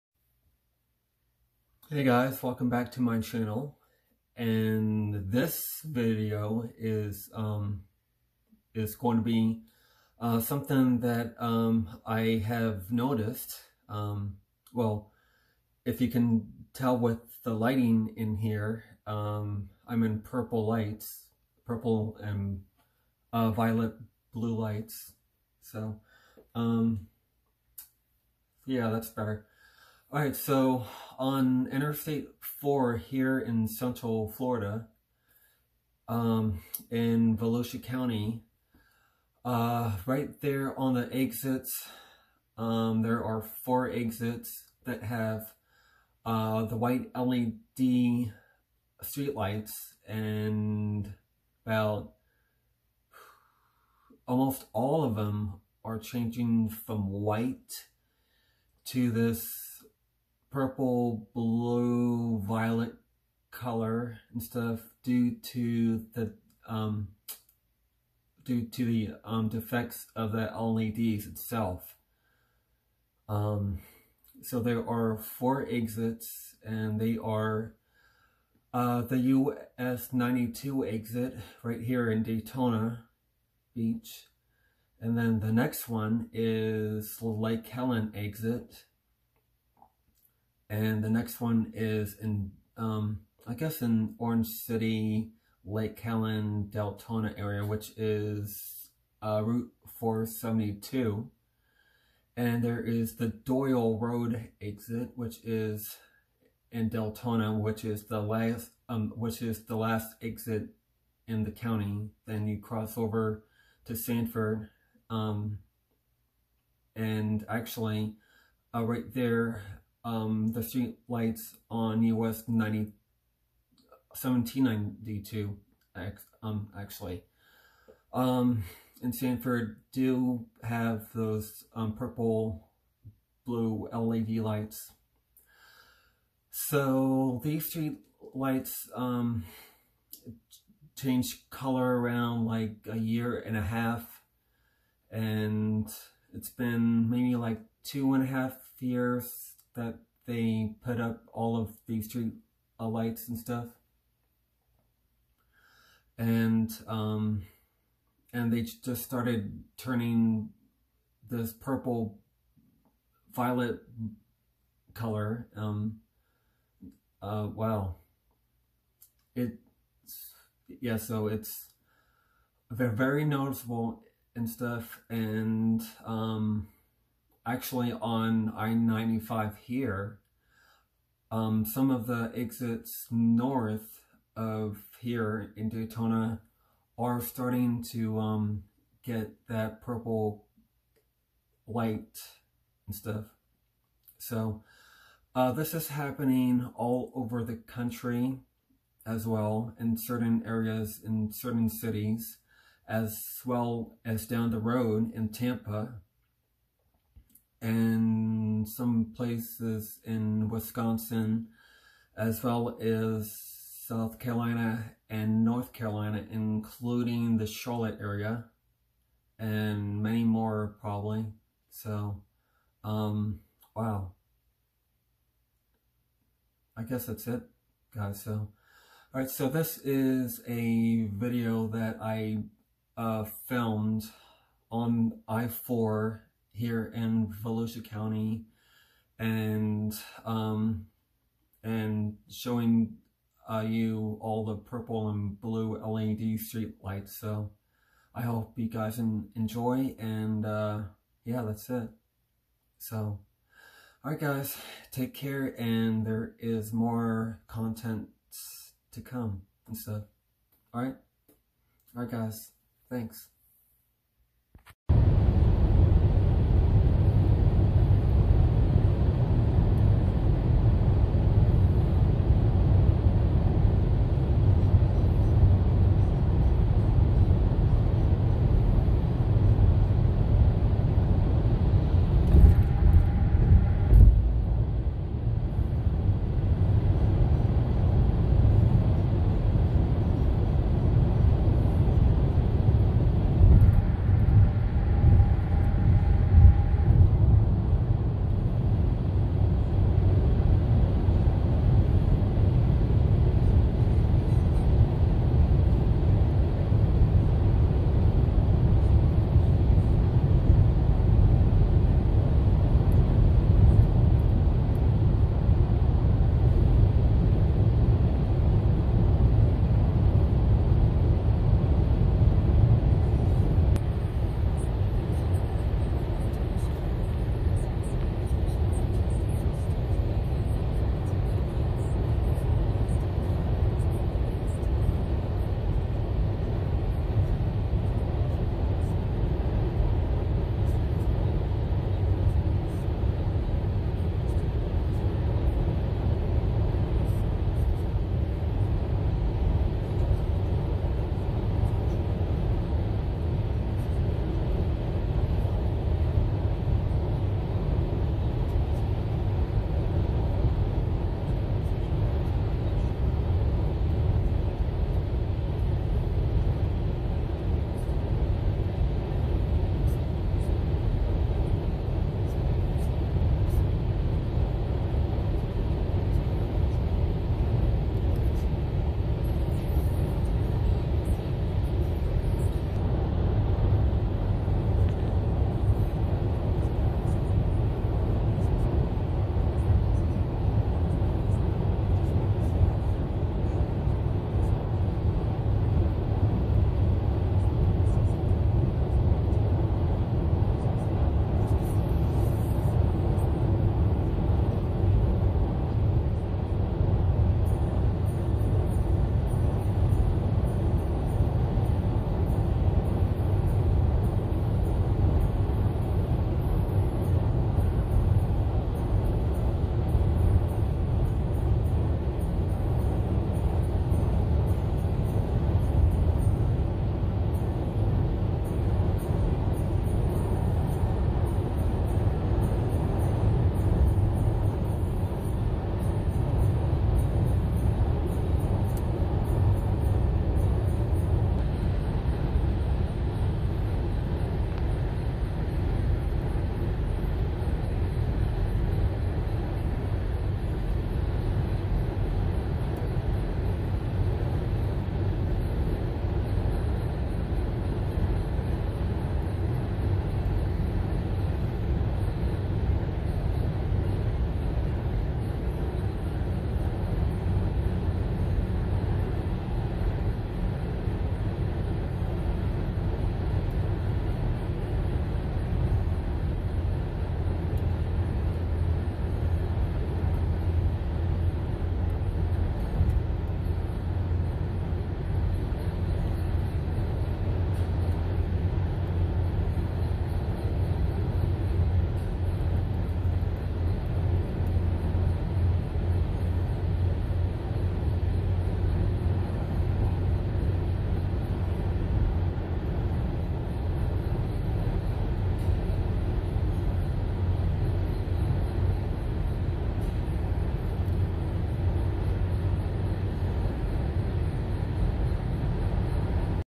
Purple streetlights on Interstate 4
💡 LED streetlights are changing from white to blue and purple violet colors on all of the exit ramps on Interstate 4 here in Volusia County, Florida. It started around a year and a half ago and they put the streetlights up around 2 1/2 years ago. In other areas in the country some LED streetlights are changing from white to blue purple as well. This is due to the lights being defective with the phosphors going bad. They will replace the lights when they get the chance. In the meantime enjoy the blue and purple LED streetlights.💡